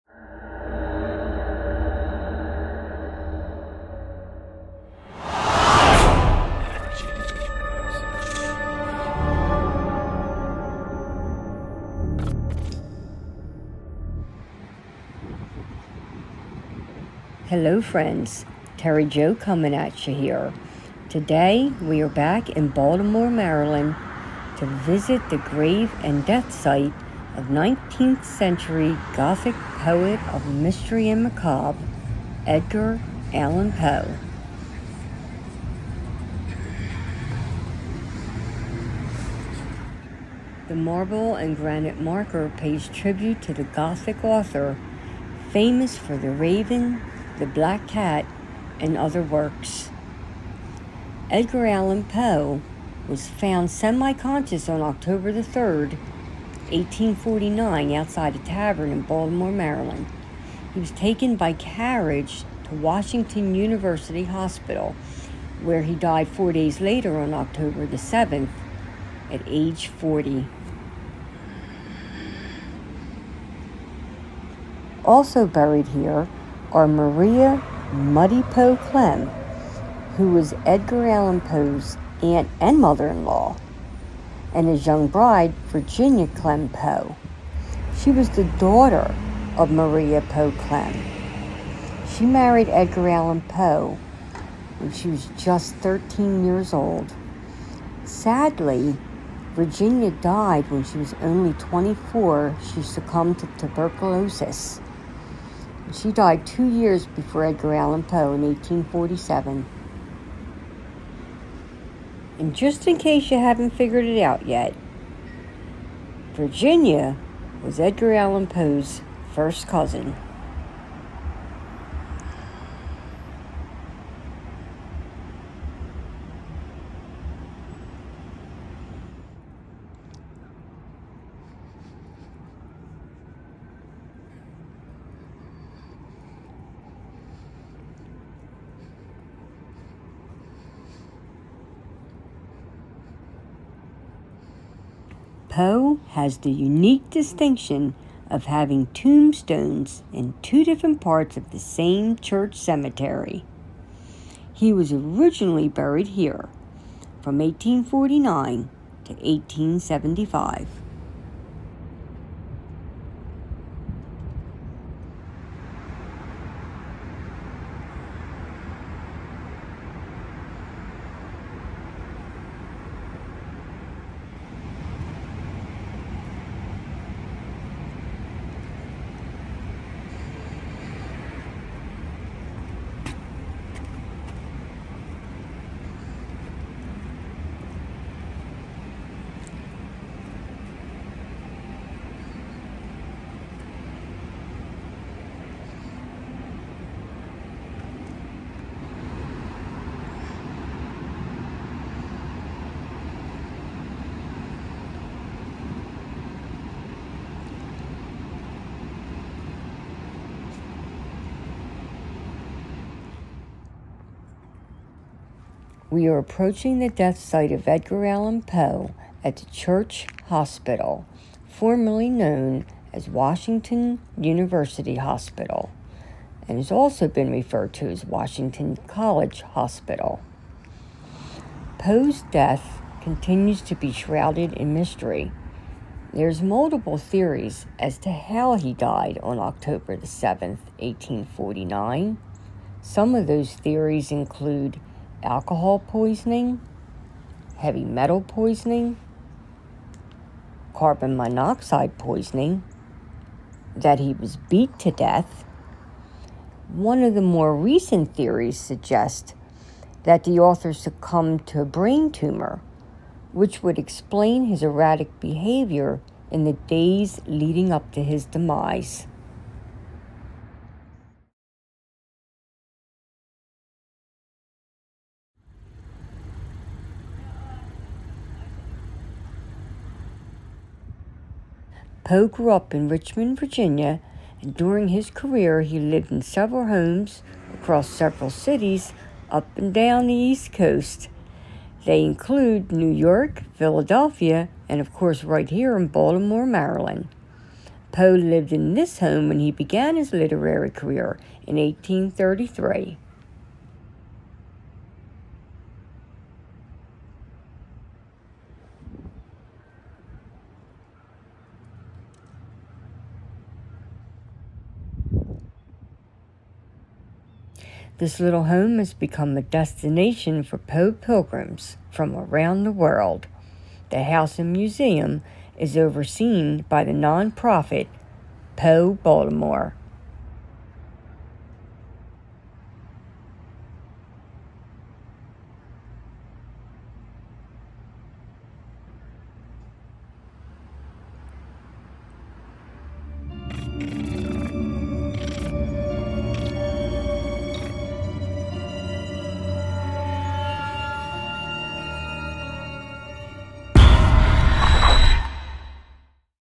Edgar Allan Poe’s Grave & Death Site
This video was filmed on January 28th, 2023 @ Westminster Hall in Baltimore, Maryland. This video features ￼Edgar Allan Poe‘s, grave, death site, and his Baltimore residence.